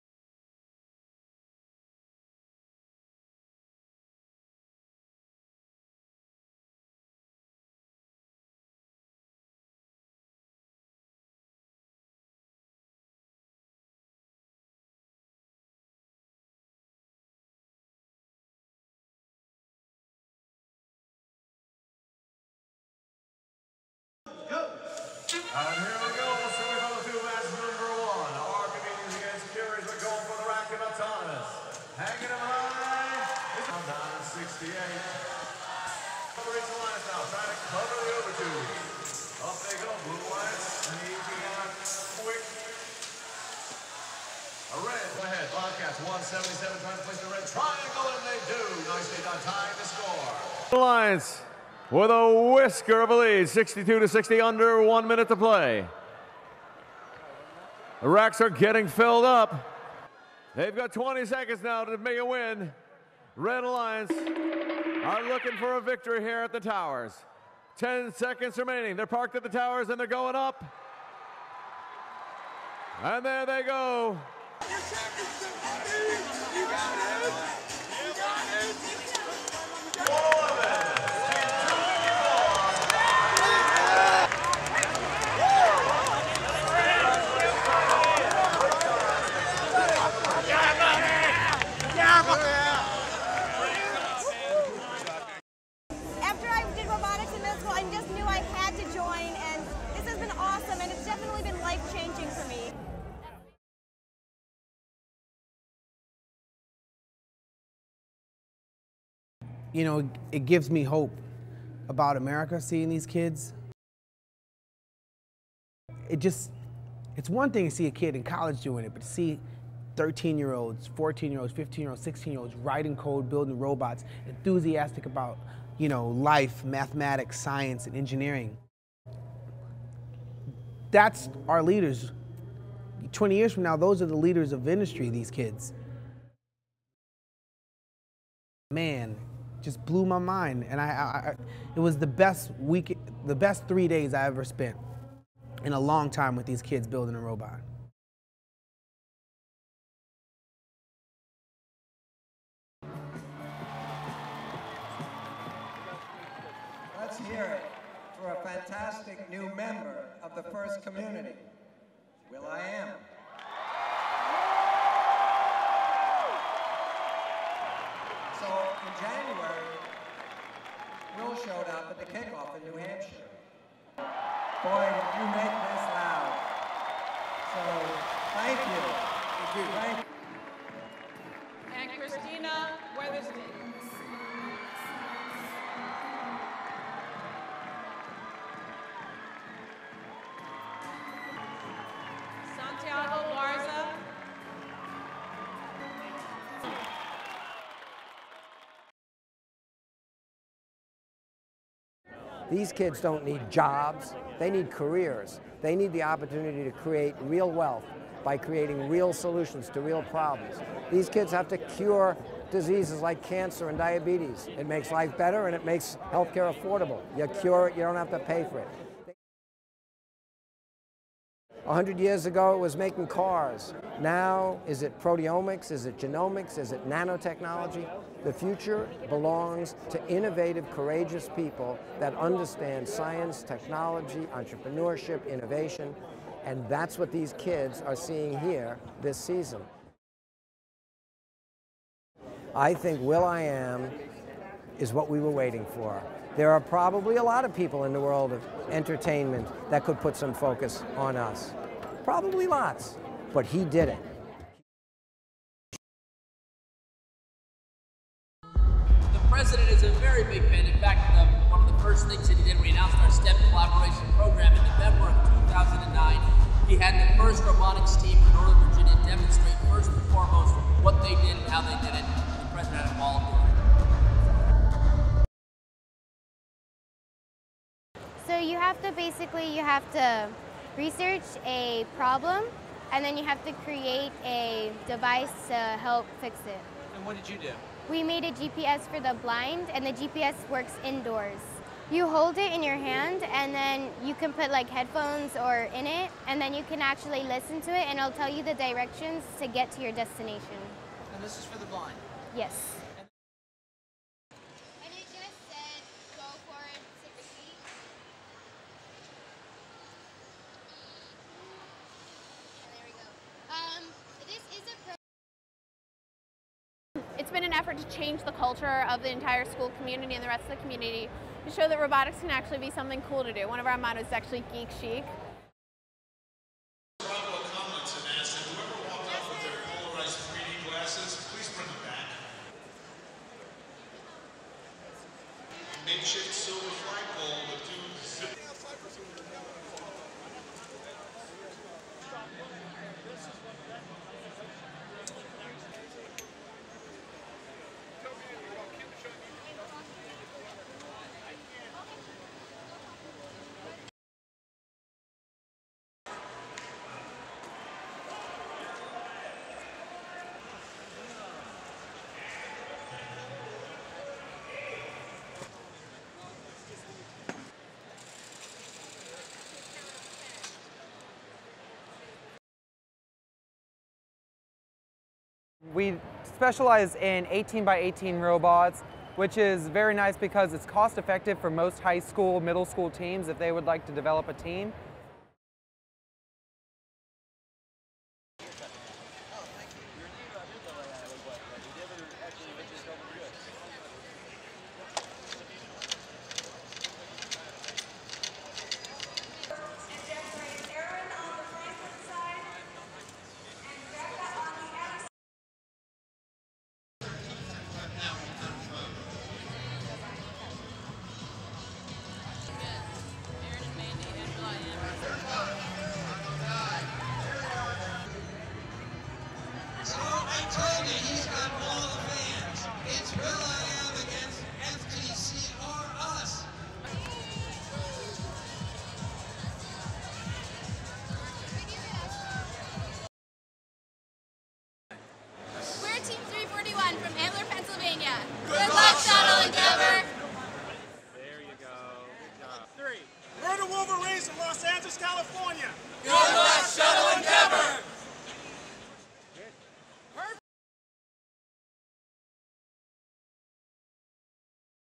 2011 FIRST® Championship Celebrates Science
Technology and Teamwork With will.i.am and White House Technology Officer